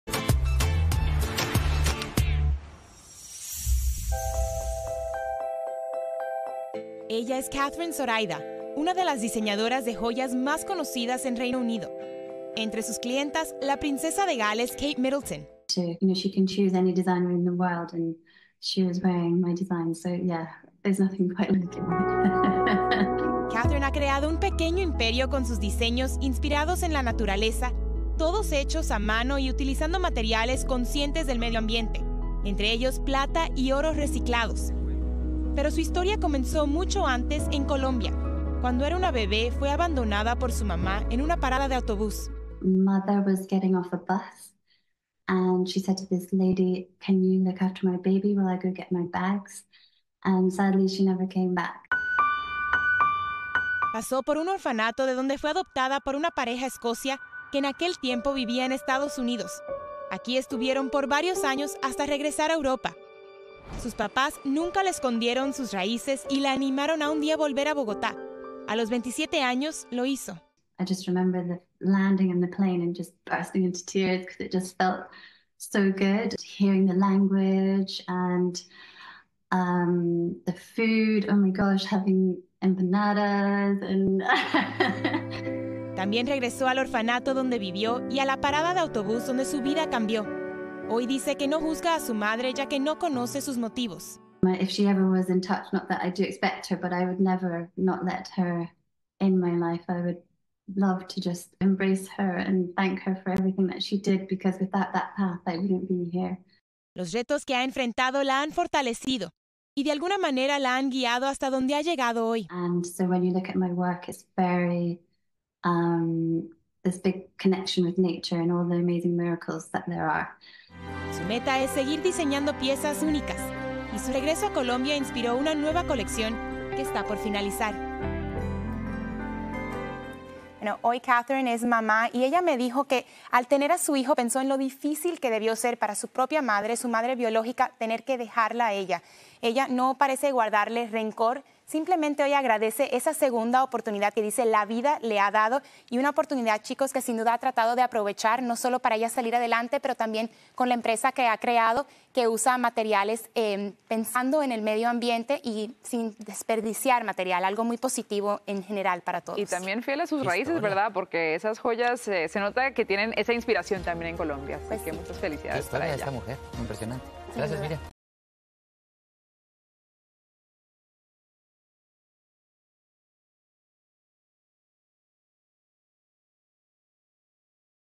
Colombiana diseña joyas en Reino Unido y Kate Middleton es una de sus clientas | Noticias Telemundo
Video oficial de Noticias Telemundo. Catherine Zoraida nació en Colombia, fue abandonada en una parada de bus, llevada a un orfanato y luego fue adoptada por una pareja escocesa. Hoy es una de las diseñadoras de joyas más reconocidas de Reino Unido.